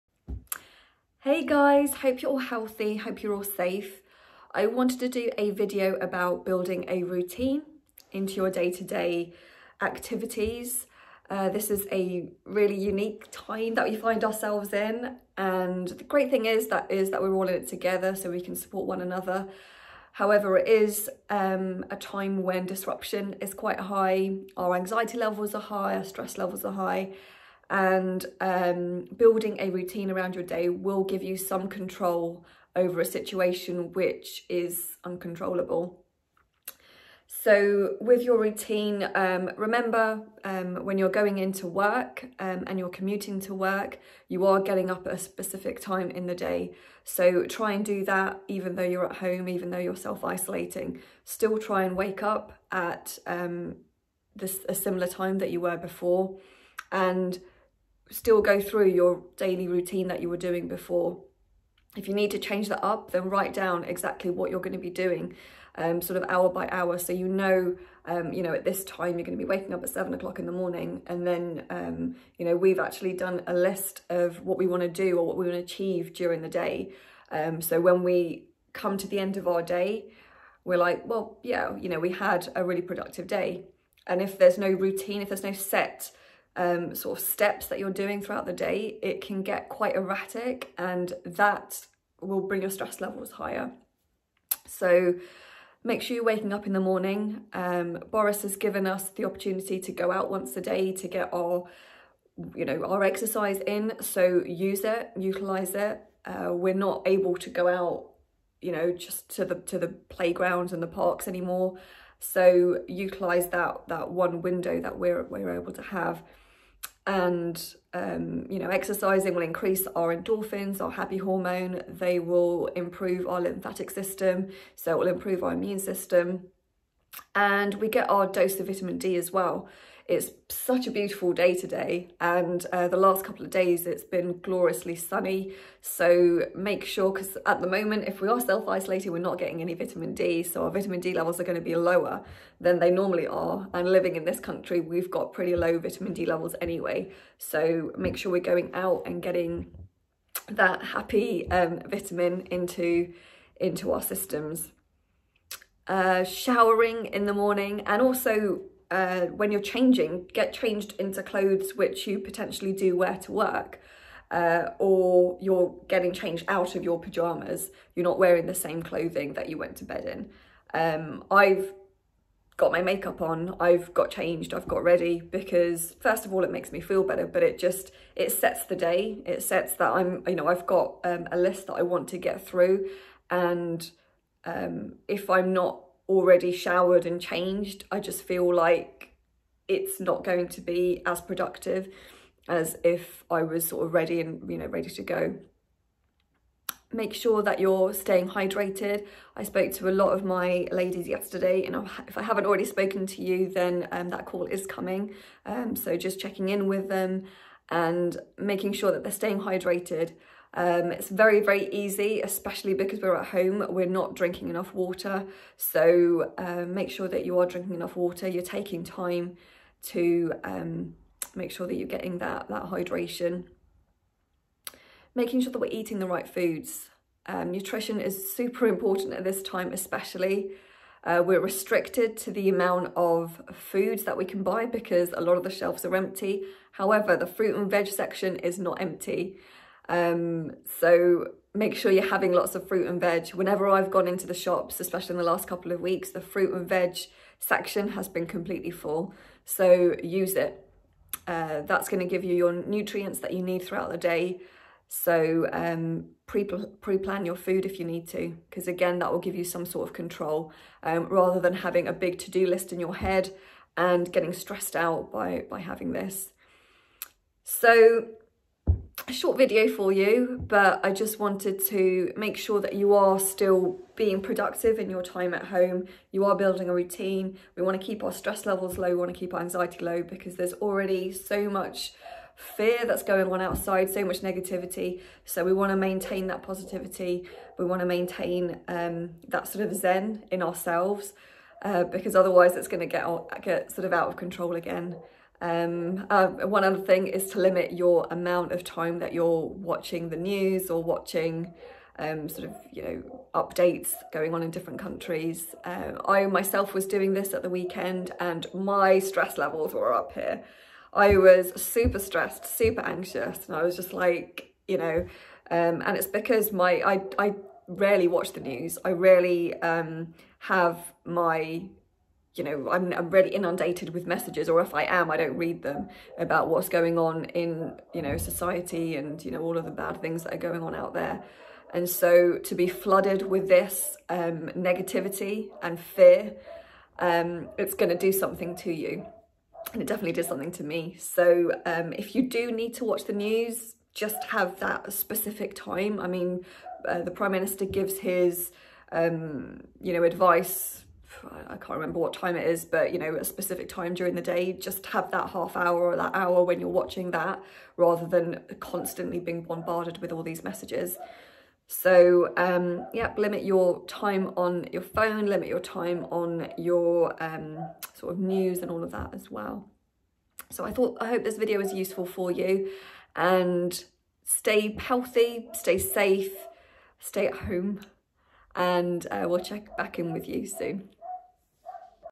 Building A Healthy Routine During COVID-19
We are in a really unique situation and the great news is that we are all in this together
We may be feeling higher levels of stress and anxiety
Developing a daily routine can help us to feel more in control of everything
The certainty of our routine can help us to manage the uncertainty with everything we are currently faced with
When we have more structure to our day, coping with the unpredictability is more manageable
We can see this time as an opportunity to rethink our daily lives and come up with a fresh routine which enables us to build in the more important things
Here are a few thing which you can build into your daily healthy routine
😇 - Wake up
😇 - Have a nutritious breakfast
😇 - Go out for some fresh air and exercise
😇 - Shower and get dressed (if make-up makes you feel better, slap it on!)
😇 - Time allocated for working - if working from home, schedule breaks, if not ‘working from home’ schedule time for what is important for you (reading a book you have previously not had time for; sorting out the wardrobes; decorating)
😇 - Lunch - you can batch prepare your meals, so you have them ready for you (which again can be scheduled)
😇 - Family time; connecting in with friends virtually
😇 - Maybe a bit more work
😇 - Stretching/yoga/meditation
😇 - Reading/TV/ watching a film
😇 - Getting ready for bed
😇 - Sleeping well

Pay attention to how you feel each day, how you all get along, and what you get done
This will be a work in progress and may change day to day - so give yourself the permission to be flexible with it
Pay attention to how it feels to be less scheduled so we are free to determine our days
Moving forwards, this will hopefully shape a life that has a little more breathing room than before